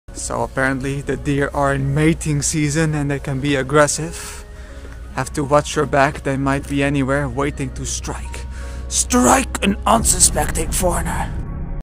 Deer Sneak Attack!
These deer are waiting to STRIKE an unsuspecting foreigner!